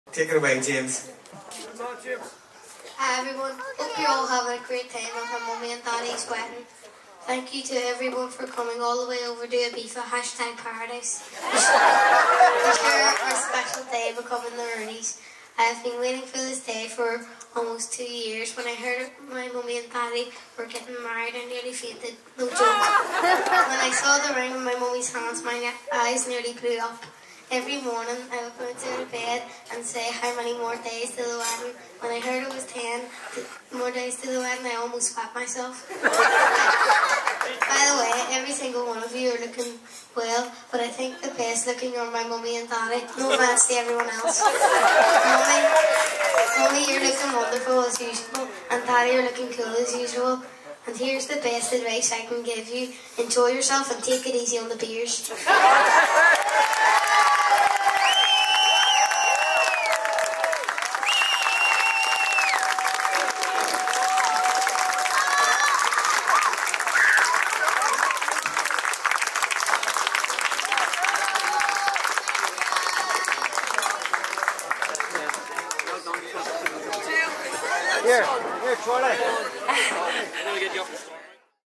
Bride's son gives cutest speech at wedding in Ibiza
How cute is James giving a speech at his mummy and daddies wedding? We love capturing priceless moments like these 😍